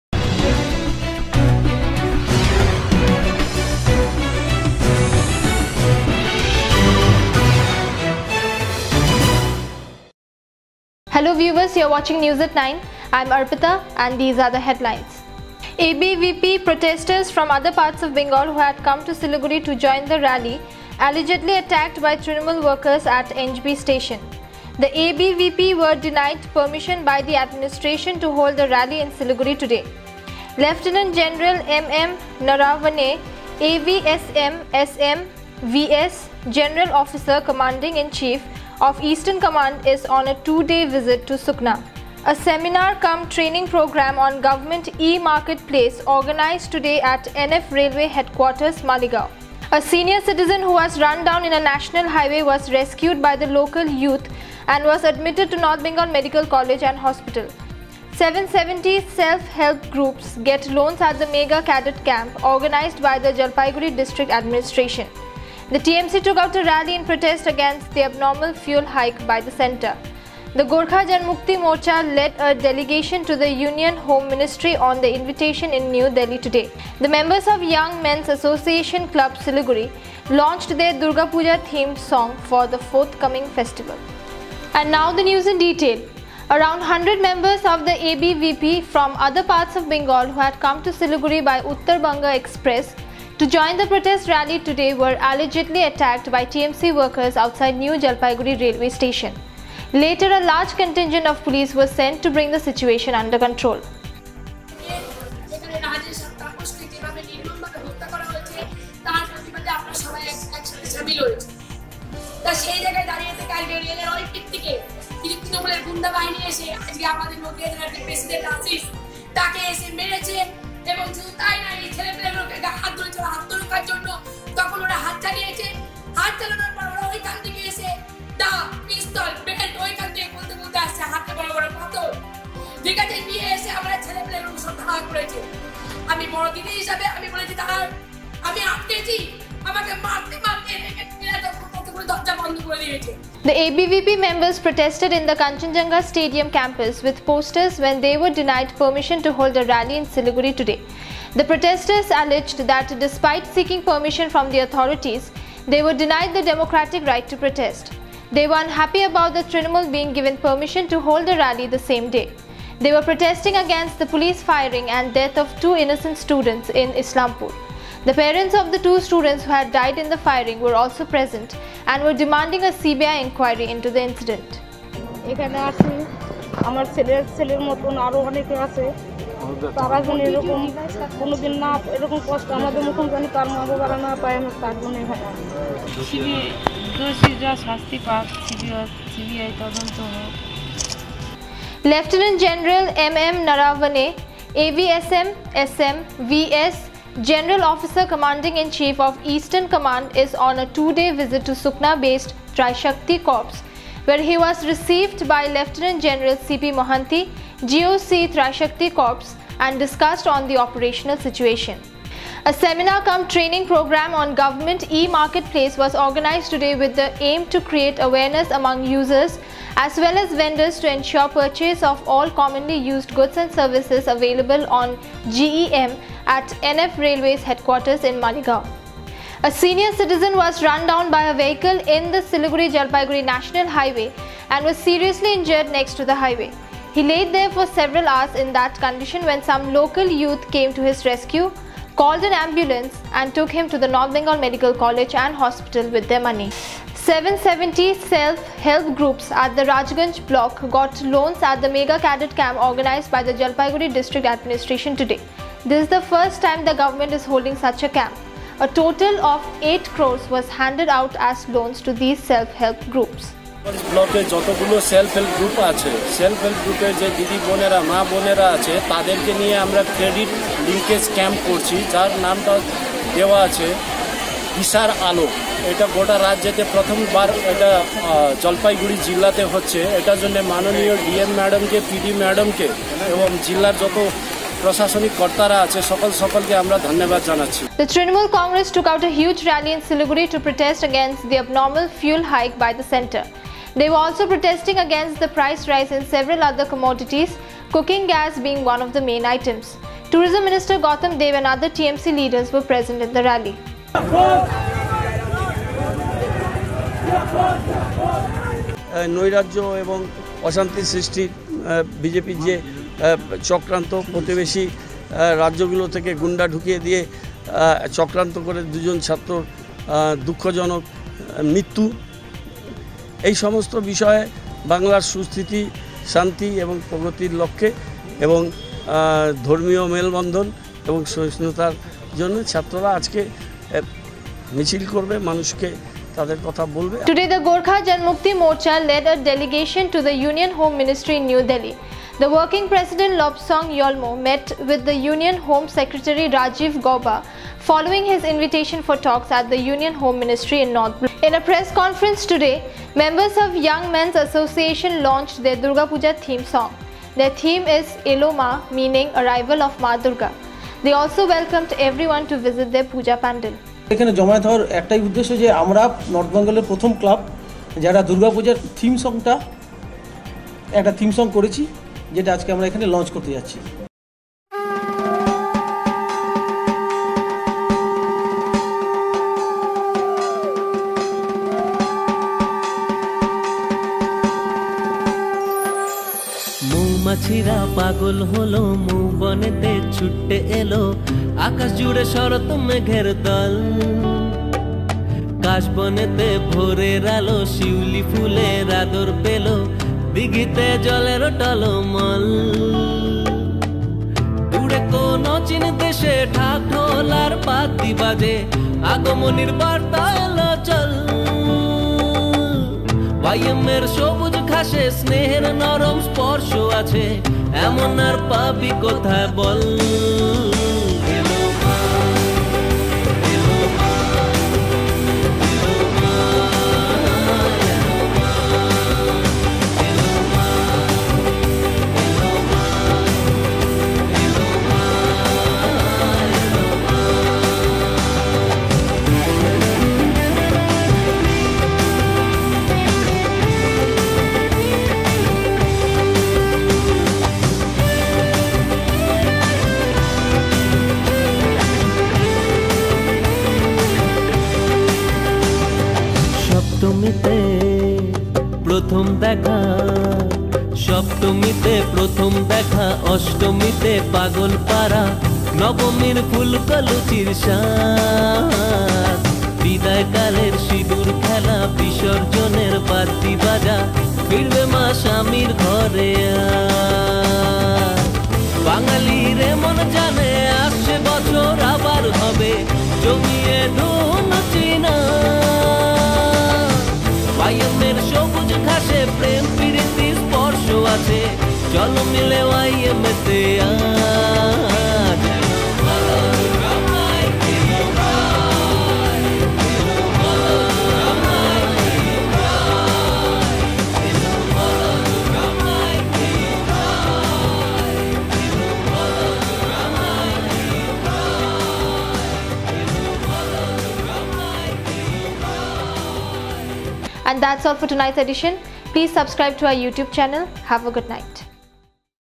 ABVP protest
ABVP protestors from other parts of Bengal who had come to Siliguri were allegedly attacked by Trinamool workers at NJP station, The ABVP were denied permission by the administration to hold a rally, Lieutenant General MM Naravaneis on a two day visit to Sukna, A seminar cum training  training programme on Government E-market place organised today at N.F railway headquarters,Maligaon, A senior citizen was run down in a National Highway was rescued by the local youth, 770 self help groups get loans at the Mega Cadet Camp organised by the Jalpaiguri District Administration, The TMC took out a rally against abnormal fuel hike by the Centre, The Gorkha Janmukti Morcha led a delegation to the Union Home Ministry of their invitation in New Delhi, The members of Young Mens Association club launched their Durga Puja theme song for the forthcoming festival